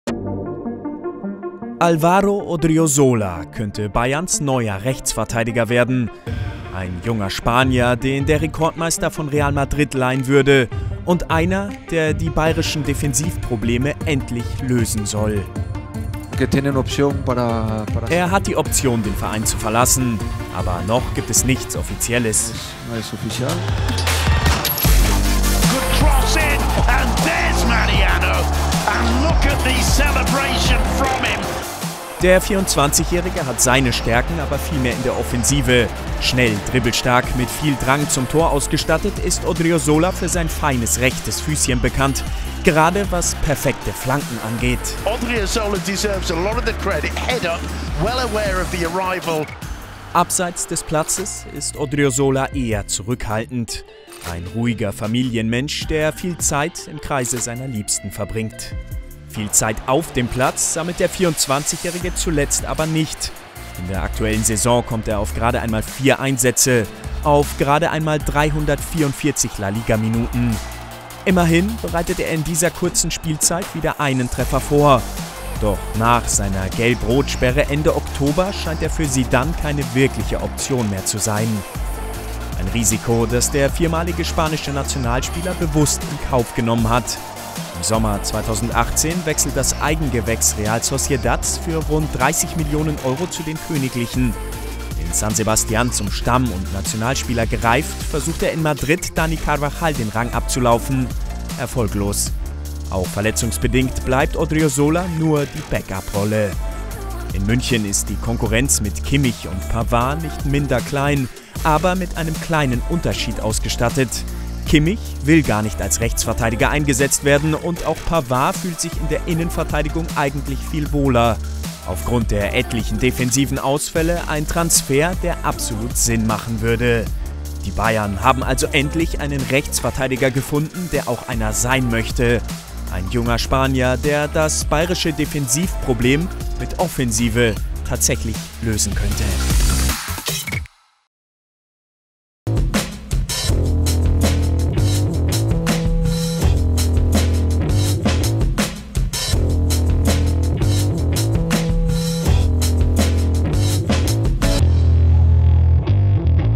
Odriozola-Deal vor Abschluss: So gut ist Bayerns neuer Rechtsverteidiger | SPORT1
Der FC Bayern steht kurz vor der Verpflichtung von Real-Profi Álvaro Odriozola- Das sind seine besten Szenen in La Liga.